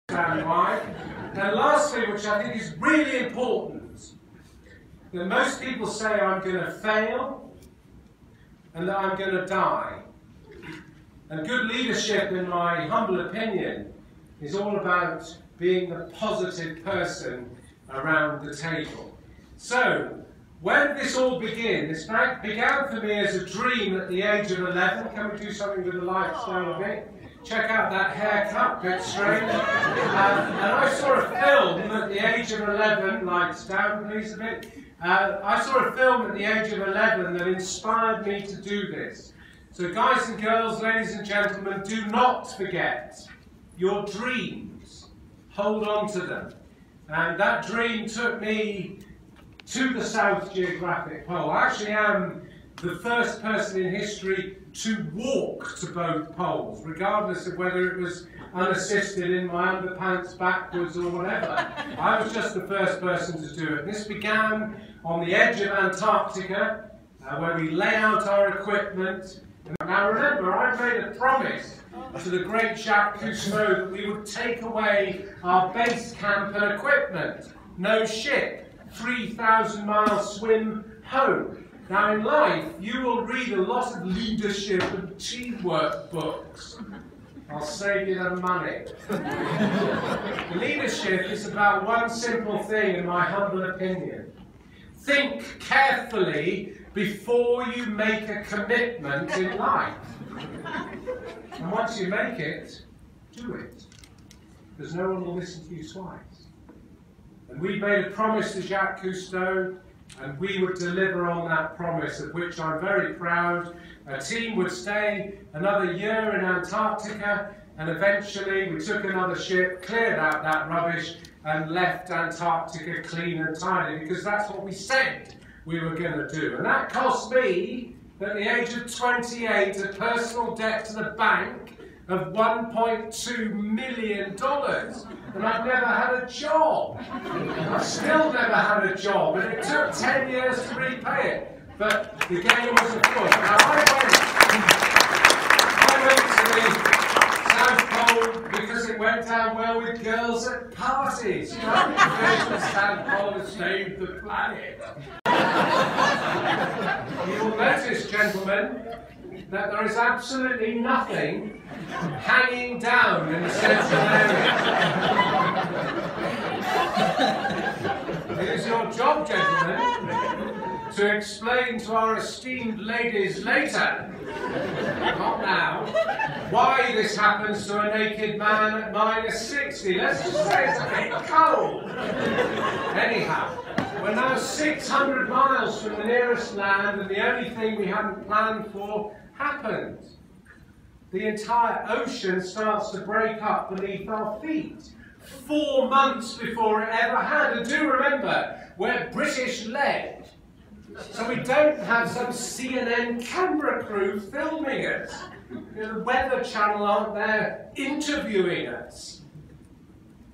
Robert Swan at One Young World Environment Summit
Robert Swan was the first man to walk on North and South Pole unassisted which he changed the world.